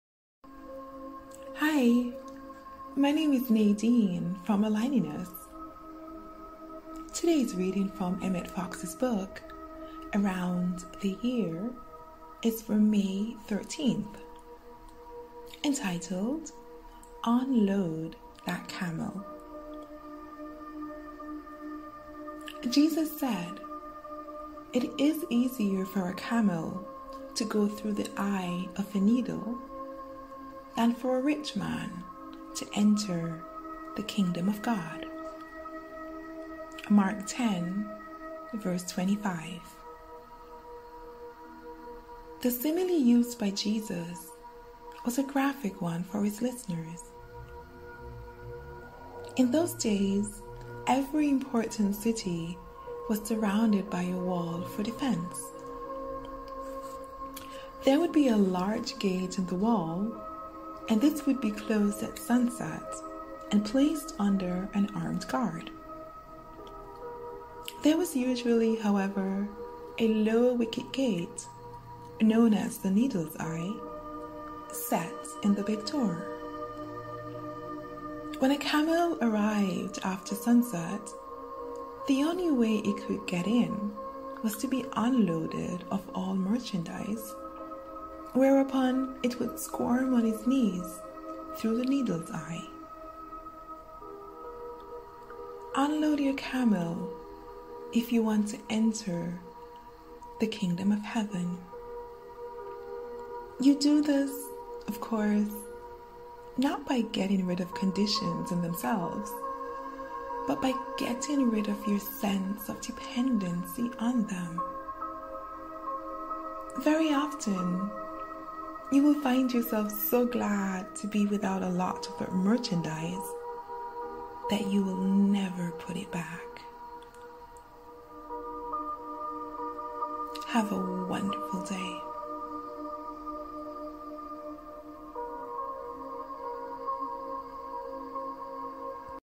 Emmet Fox - May 13 ✨ Unload That Camel - Around the Year
A daily reading from Emmet Fox's book Around the Year ✨

Nourish your Soul 365 Days a year 😍